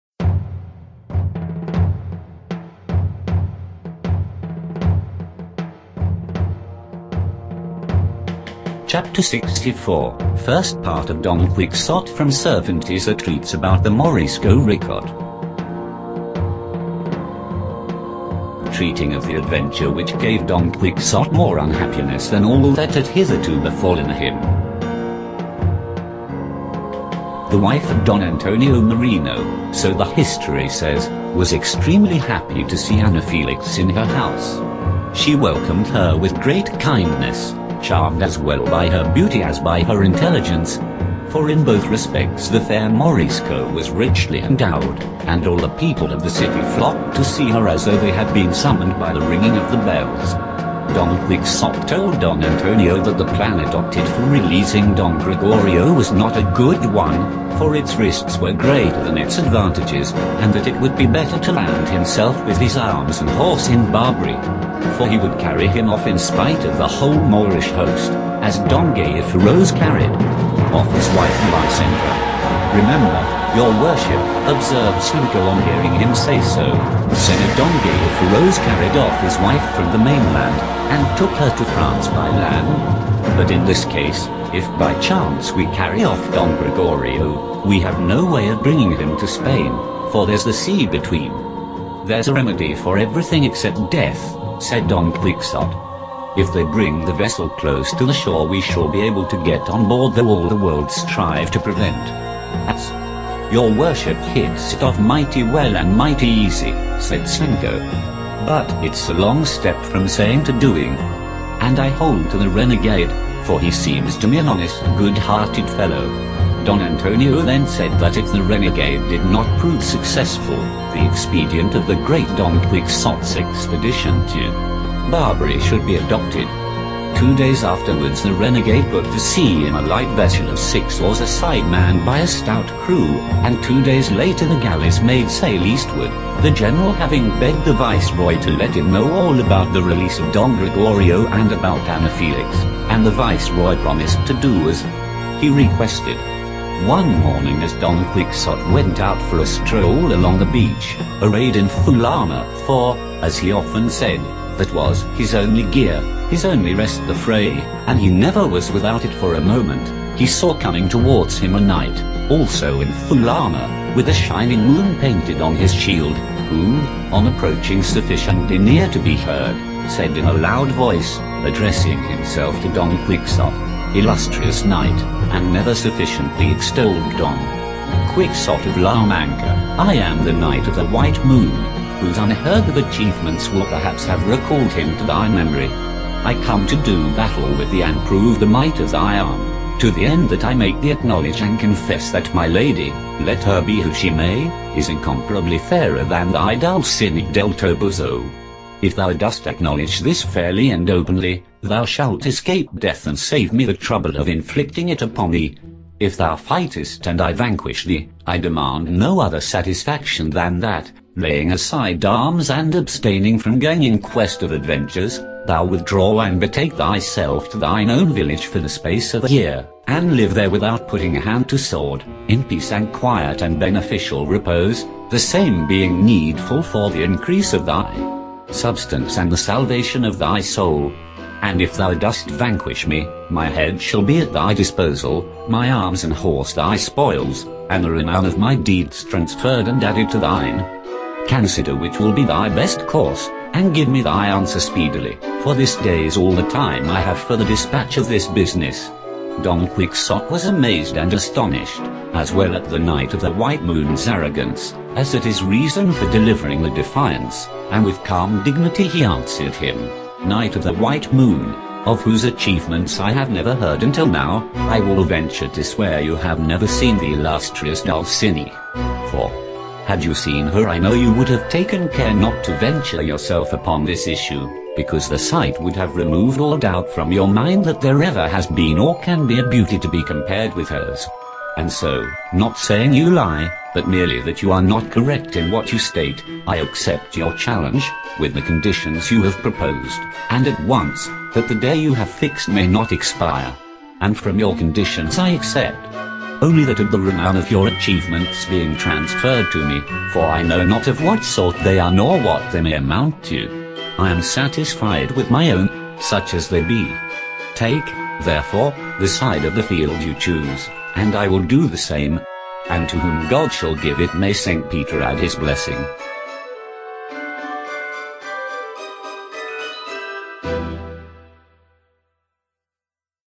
Morisco Ricote of the Ricote Valley 64-1, Chapter of Cervantes.wmv
Miguel Cervantes  de Saavedra deals the  Morisco Ricote in his work:    " Don Quijote de la Mancha", book II of the year 1615  in the chapters 54, 55, 63, 64 y 65. The story of Miguel Cervantes de Saavedra is based on real events. Cervantes had his information from his friend Juan de Urbina, secretary of the Prince Emmanuel Philibert of Savoy. The  Prince  Emmanuel  Philibert of Savoy was  general at sea and in charge with the expulsion of the Moriscoes of the Ricote Valley, at the end of 1613, through the harbor in Cartagena. The tragedy of the expulsion of the Moriscoes of the Ricote Valley occurred in the six villages of the Valley, namely:      Abarán, Blanca, Ojós, Ricote, Ulea  and Villanueva.
This video deals chapter 64, first part, with photographs of Ojos.